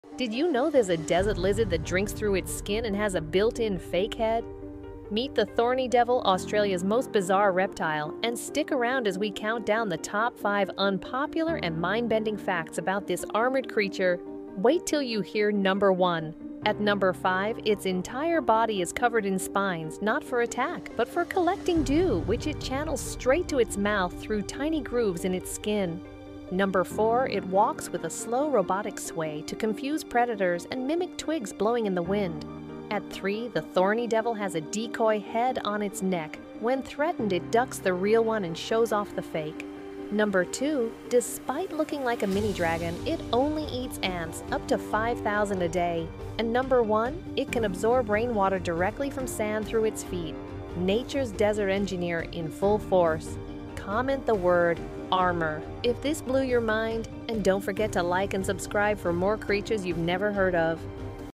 This Lizard Drinks Through Its Skin? Meet the Thorny Devil
Have you ever seen a lizard that drinks with its skin, walks like a twig, and has a decoy head? Meet the Thorny Devil, one of the most fascinating and underrated reptiles in the world. Native to Australia’s scorching deserts, this spiky little survivor has evolved some of the most mind-blowing adaptations in the animal kingdom.

In this video, we count down 5 unbelievable facts about the Thorny Devil you’ve likely NEVER heard before. From absorbing water through its feet to tricking predators with a fake head, this lizard is proof that nature never runs out of surprises.

👍 LIKE if you enjoy learning about strange creatures
💬 COMMENT your favorite fact or just type ARMOR